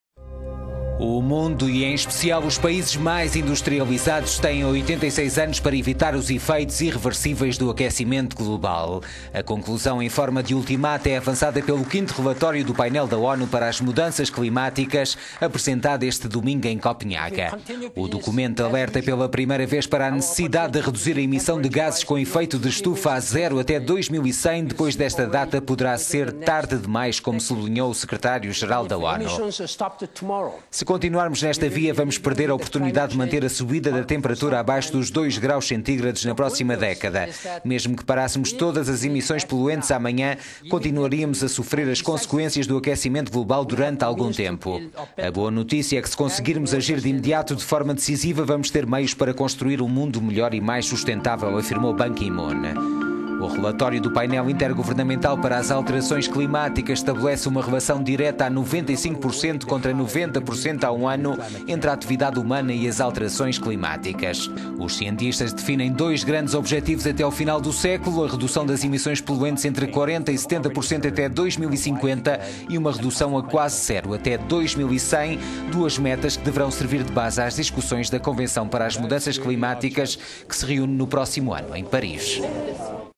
ONU lança "ultimato" climático: o mundo tem 86 anos para reduzir a zero as emissões poluentes
O mundo, e em especial os países mais industrializados, têm 86 anos para evitar os efeitos irreversíveis do aquecimento global. A conclusão, em forma de ultimato, é avançada pelo quinto relatório do painel da ONU para as mudanças climáticas, apresentado este sábado em Copenhaga.

O documento alerta, pela primeira vez, para a necessidade de reduzir a emissão de gases com efeito de estufa até 2100.  Depois desta data poderá ser tarde demais, como sublinhou o secretário geral da ONU: